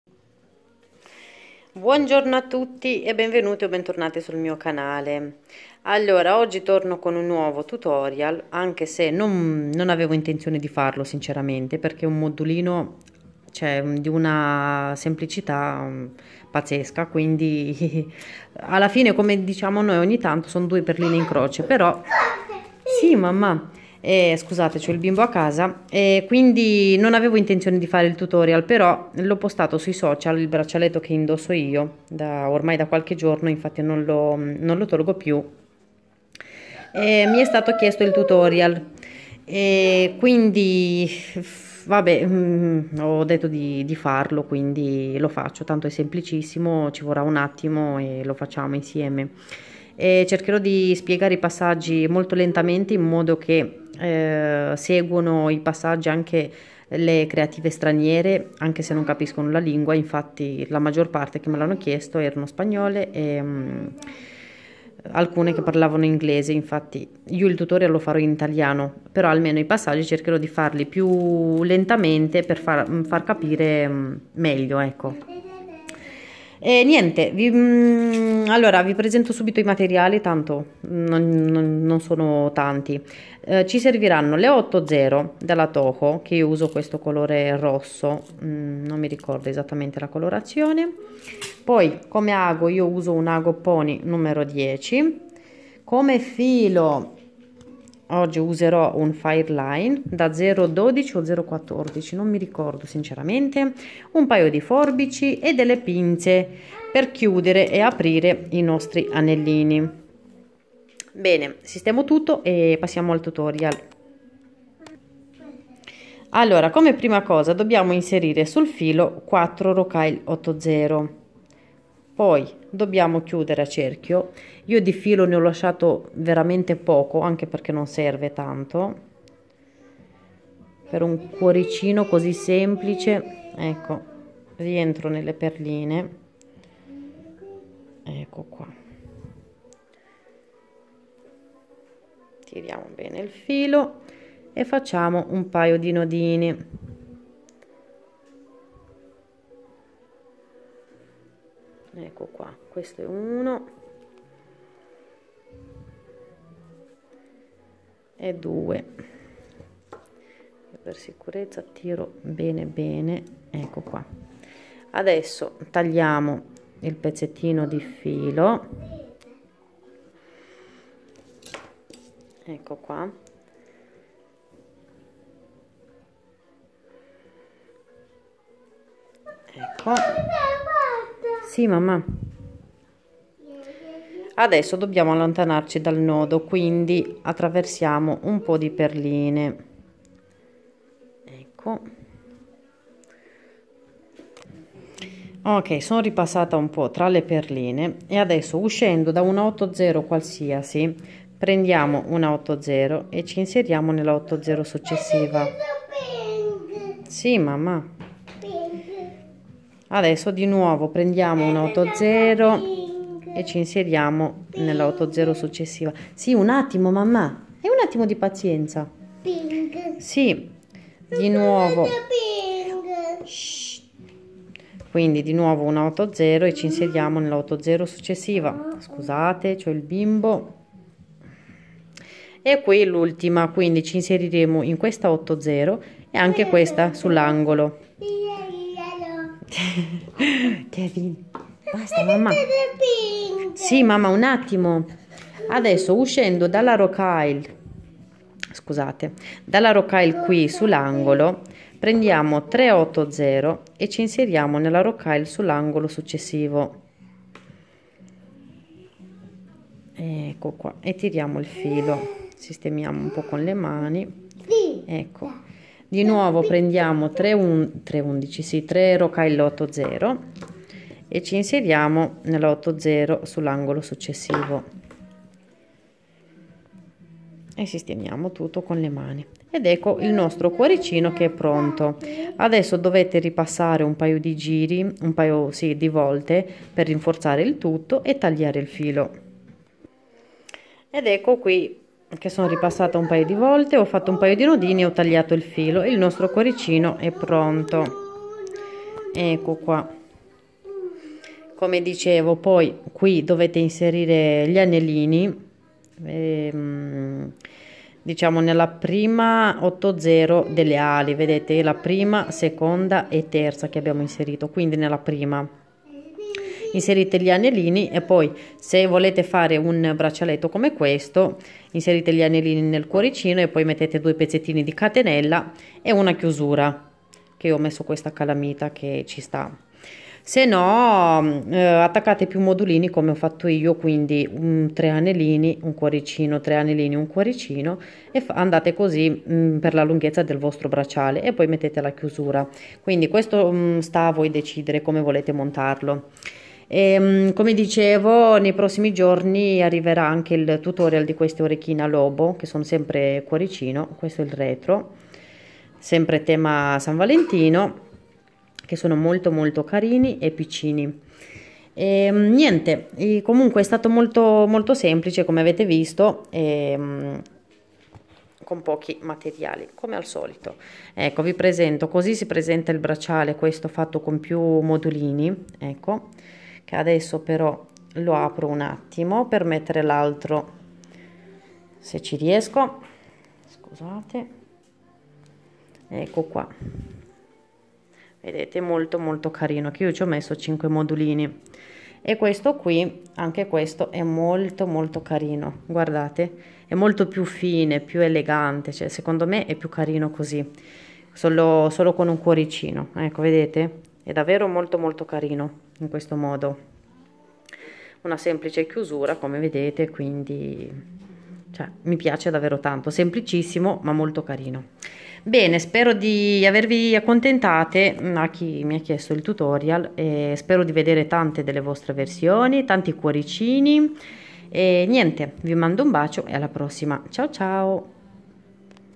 Tutorial Cuoricino, braccialetto con cuoricini, diy Heart tutorial, valentine's, cuore con perline
Ciao a tutti e benvenuti sul mio canale Youtube.
Oggi faremo insieme un modulino molto semplice , modulino a forma di cuore realizzato solo con rocailles toho 8/0.  E molto versatile in quanto si possono realizzare braccialetti,  collane,  orecchini, anelli e come charm .

Materiali/ materials
- rocailles toho 8/0(seed beads 8/0)
- ago pony n 10 ( needle pony n10)
- filo fireline 0,12 mm( tread fireline)
- anellini ( jump ring)
- chiusura ( clasp for bracelet)

Buon lavoro 😊❤

Per altri tutorial visitate il mio negozio etsy

MeryJewels23

La mia e-mail per collaborazioni